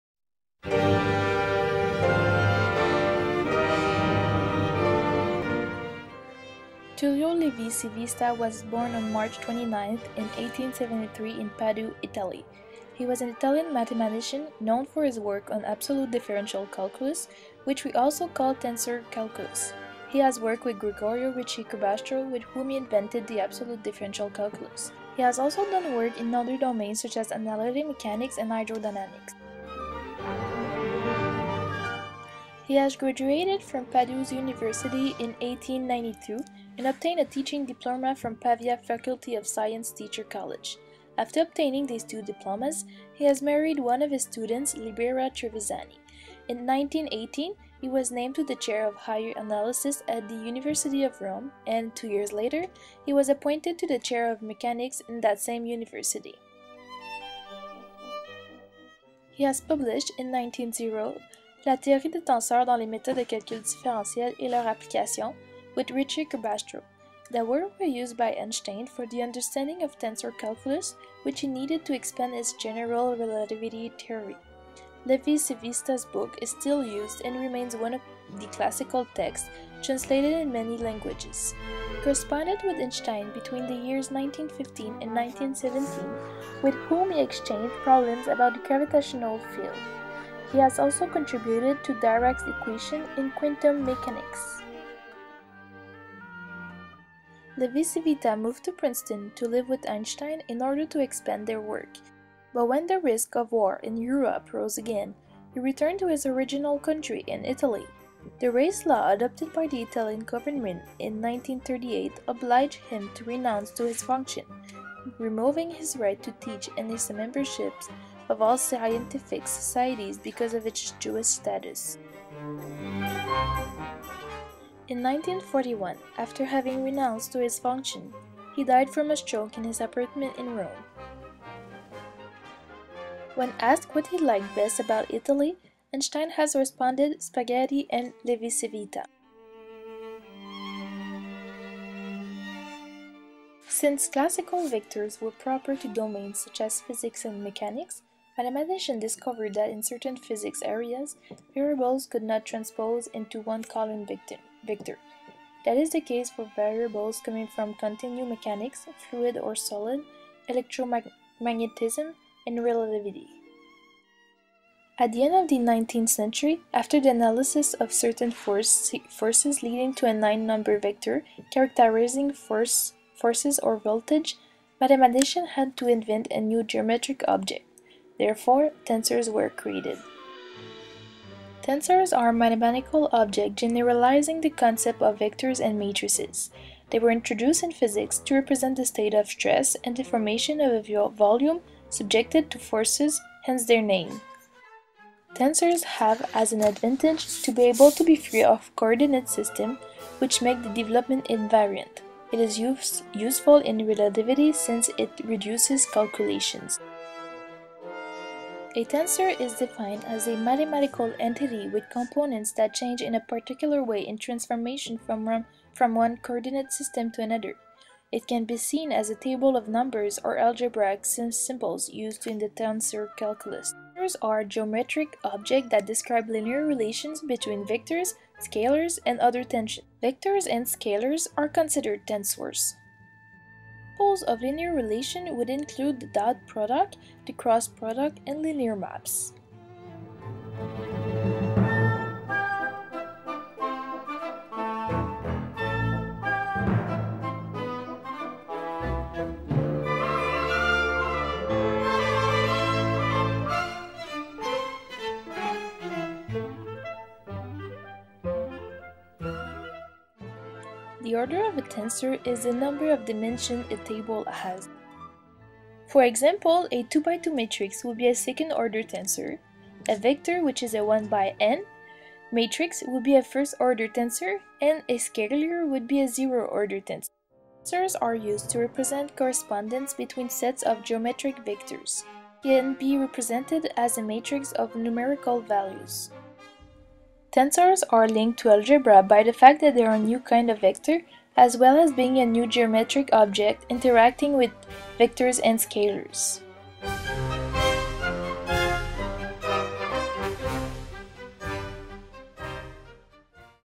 Tullio Levi-Civita
Biography of Tullio Levi-Civita and his main work, tensor calculus.